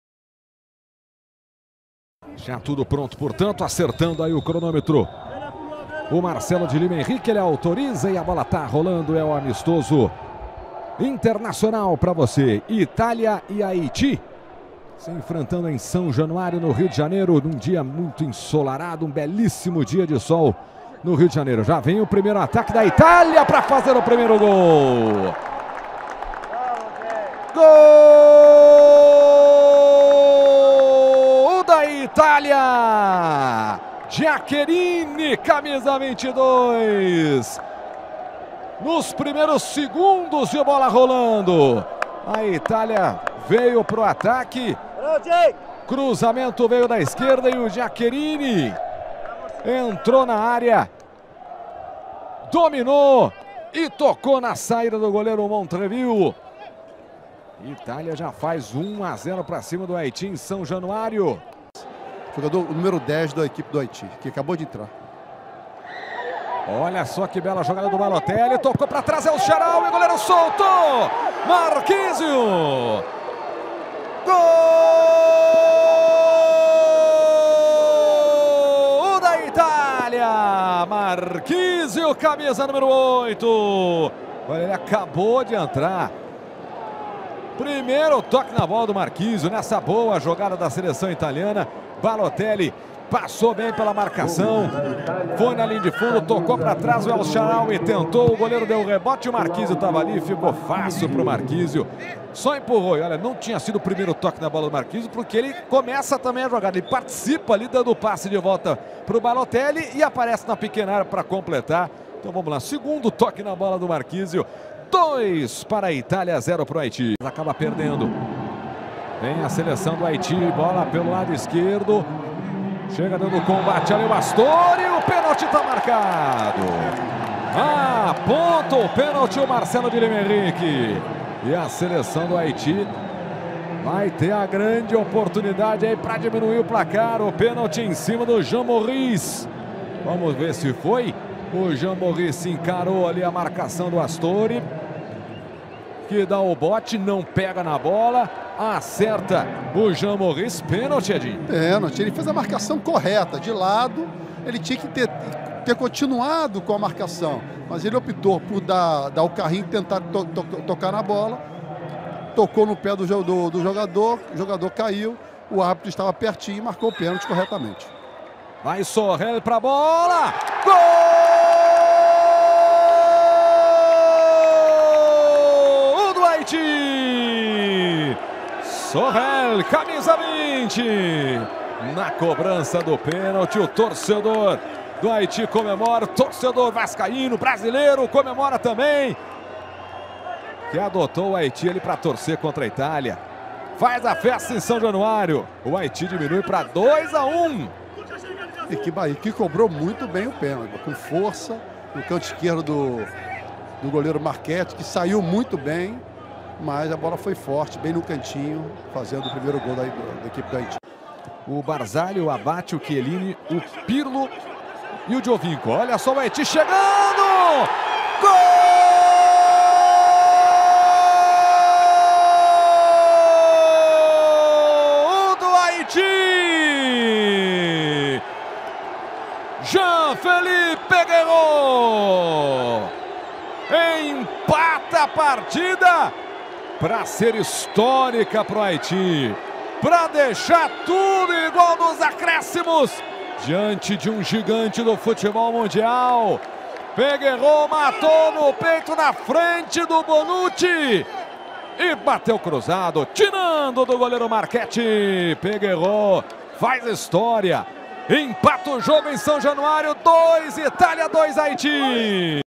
Ministère de la Jeunesse, des Sports et de l’Action Civique (MJSAC)
Italie vs Haiti (2-2) ce mardi 11 juin 2013 au stade du Club de Regatas Vasco da Gama (Rio de Janeiro) au Bresil. Les buteurs haitiens: Olrish Saurel (85e)  et Péguéro Jean-Philippe (90e)